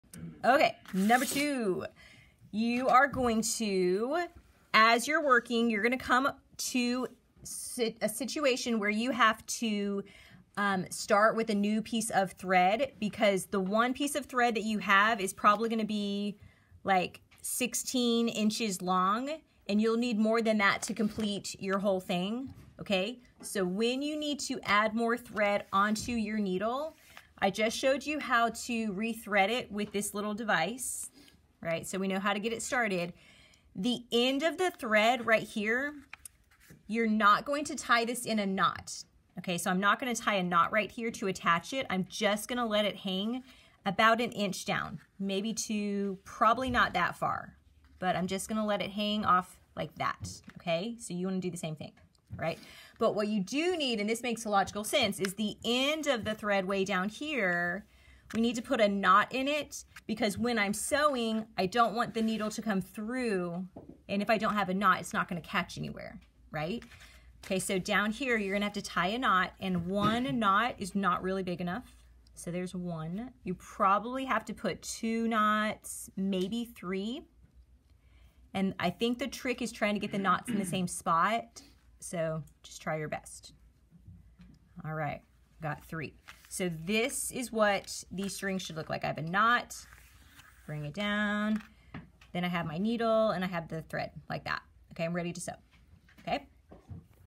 Step 2 Determining the Thread Length and Preparing the Needle for Sewing
This video answers questions about the correct length of your thread, and how to know if you are ready to sew.